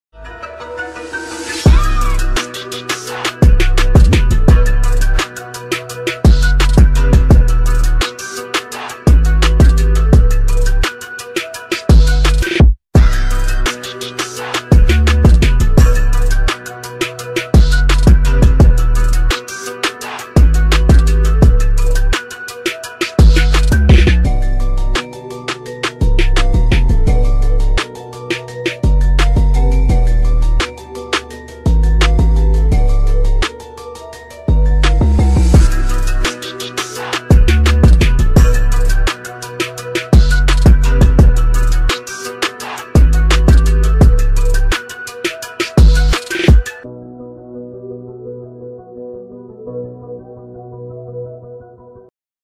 SANTIAGO TOLOZA ● Arsenal ► 2023 ᴴᴰ
● Santiago Toloza es un futbolista argentino de 20 años que actualmente se desempeña como atacante en Arsenal de Sarandi.